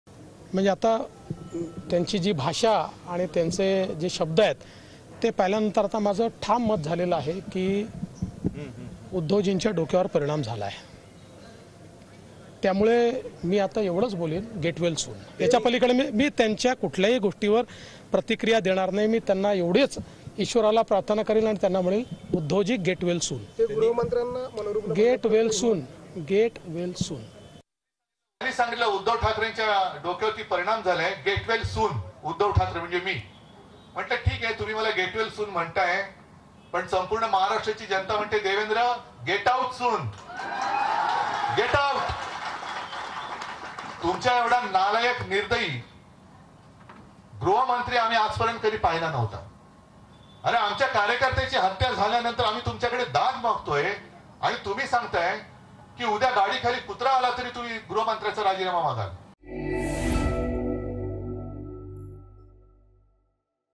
उद्धव ठाकरे आणि देवेंद्र फडणवीस यांच्यात शाब्दिक चकमक
आगामी निवडणुकांच्या पार्श्वभूमीवर शिवसेना उद्धव बाळासाहेब ठाकरे पक्षाचे प्रमुख उद्धव ठाकरे राज्यभरात दौरे करत आहेत. या दौऱ्यात उद्धव ठाकरे भाजपवर टीका करत आहेत. अशातच राज्याचे उपमुख्यमंत्री देवेंद्र फडणवीस यांनी 'गेट वेल सून म्हणत' उद्धव ठाकरेंच्या टीकेवर प्रत्युत्तर दिले आहे. तर फडणवीसांच्या टीकेला 'गेट आऊट सून' म्हणत उद्धव ठाकरेंनी प्रत्युत्तर दिले.